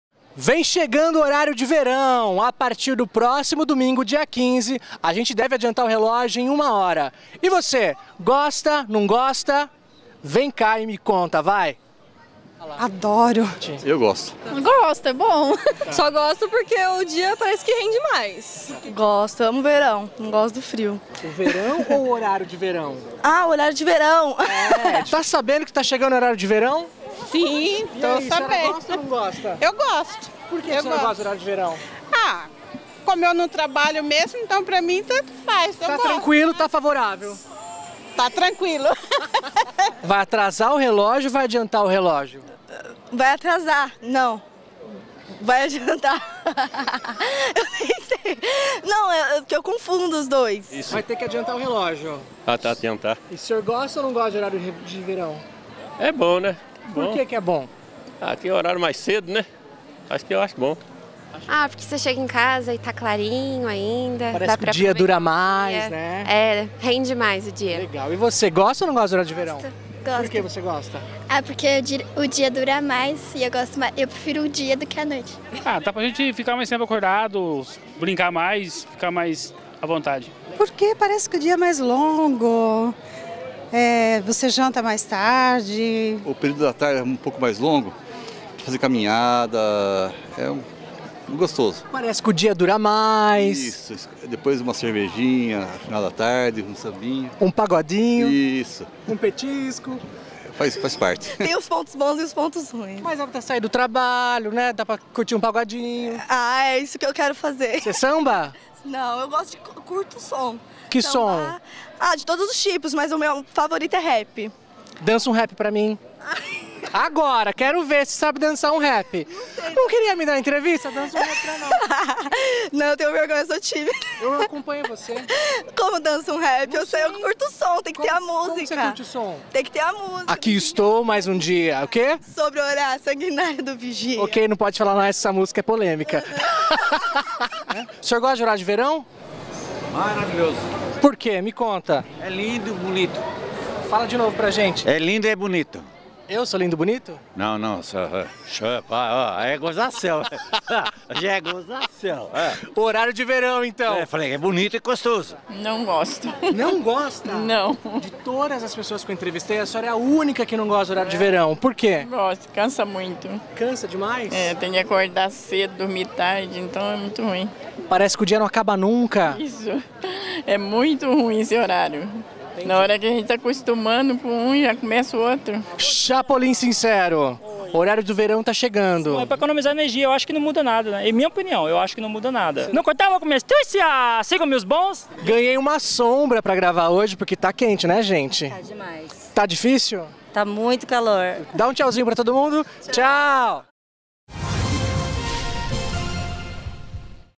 Horário de verão começou neste domingo (15)
Você gosta do horário de verão? O Anderson Muller foi às ruas pra saber o que os jundiaienses acham dessa época do ano. Confere aí!